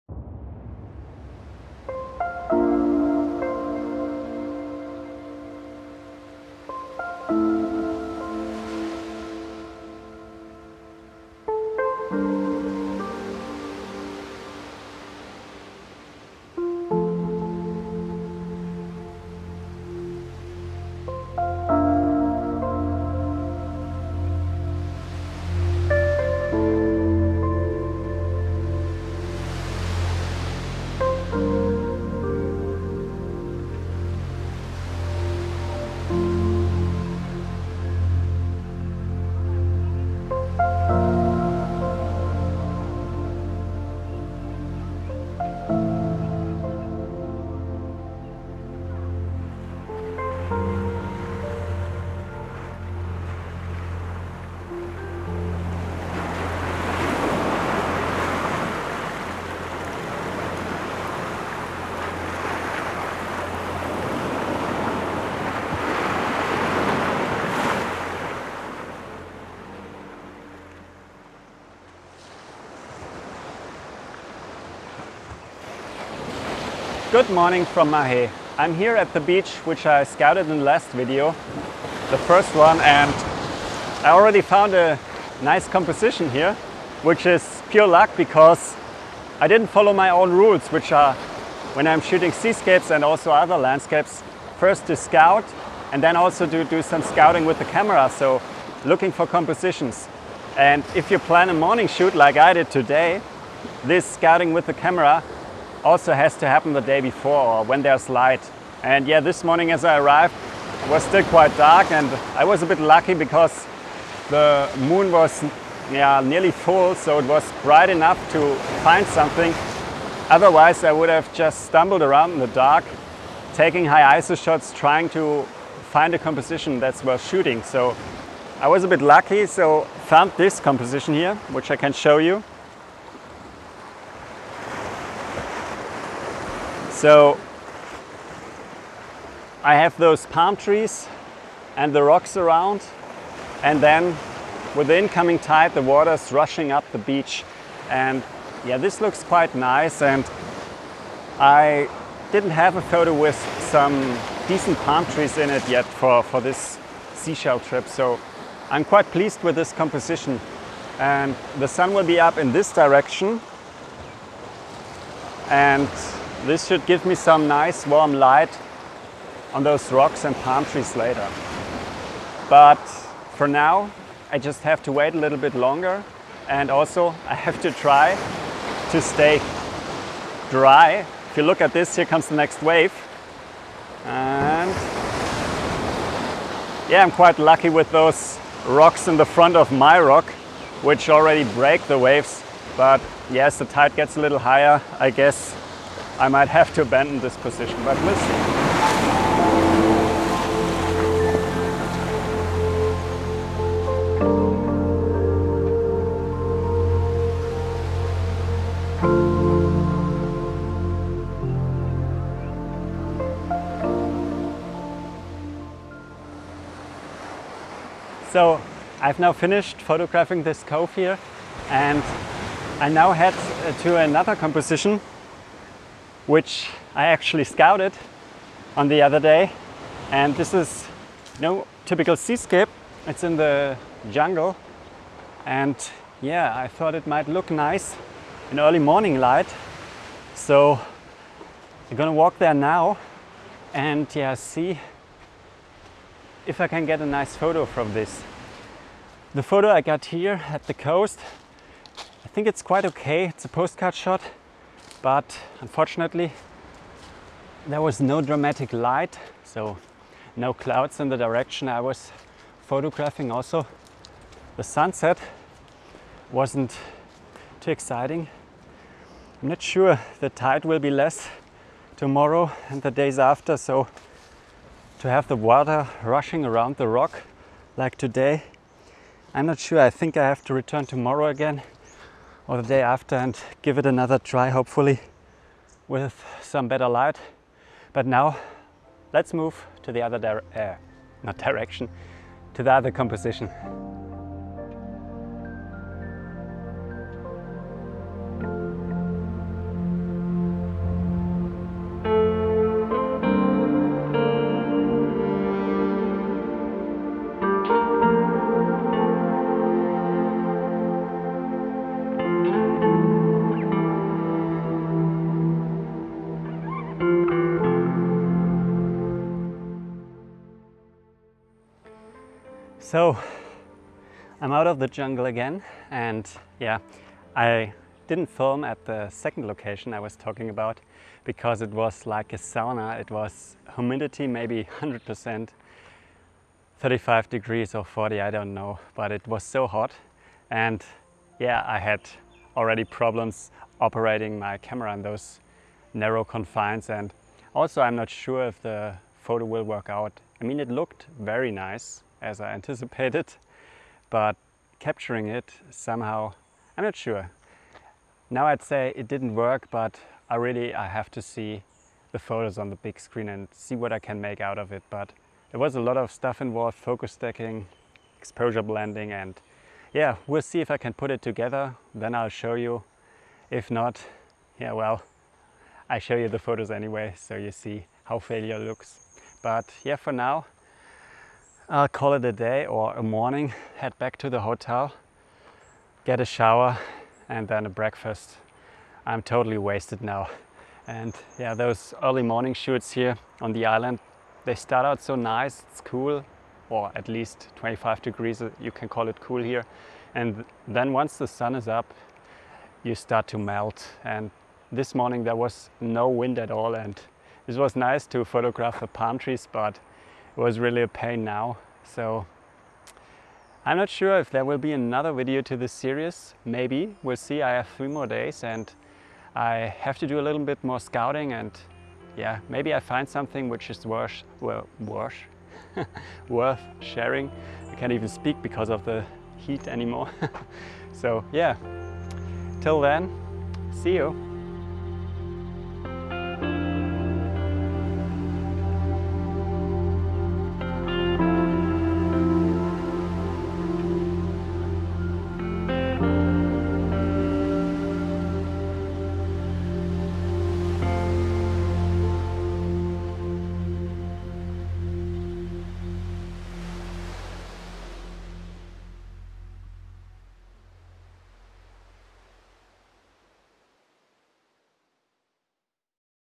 Photographing the Seychelles - Secret Beach Sunrise on Mahe
This is the last Seychelles Photography Vlog. After the jungle hike I shared in the last video, I went back to one of the beaches I had found that day, to photograph it during sunrise.

I started photographing around nautical twilight, when there were still some stars in the sky and later continued photographing until sunrise. As usual I show you my setup and share some thoughts which went into the photos. So I hope you enjoy this vlog!

★☆★ in depth video tutorials ★☆★

★☆★ follow me ★☆★

★☆★ follow me ★☆★